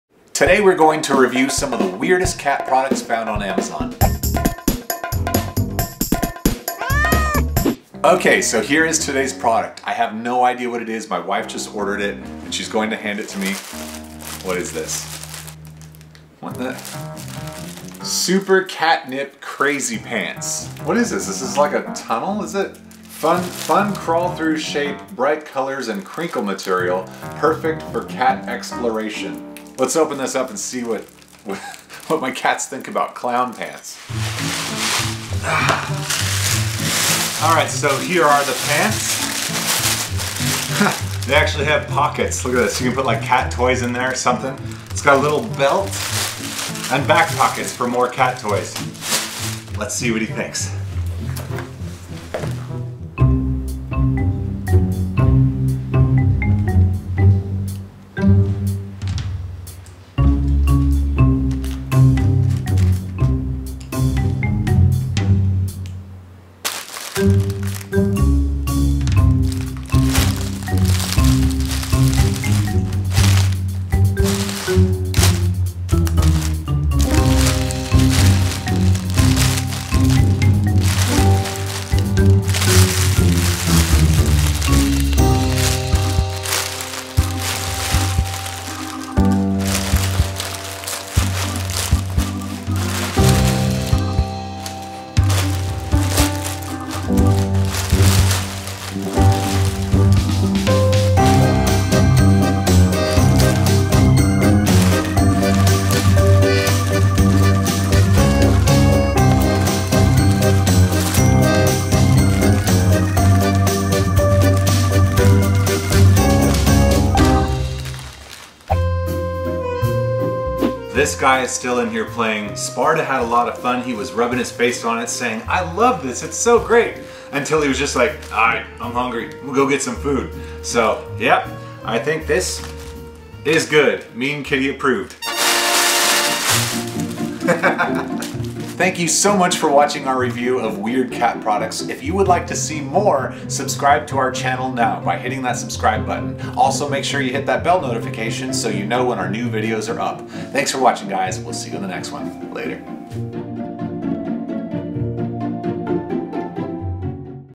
Crazy Cat Pants Review | Weird Cat Products Found on Amazon
This is our review of Super Crazy Cat Pants.  There are lots of weird cat products on Amazon, so my wife is surprising me with new crazy cat products to review each week.

Here is today’s weird cat product on Amazon and more:
Super Crazy Cat Pants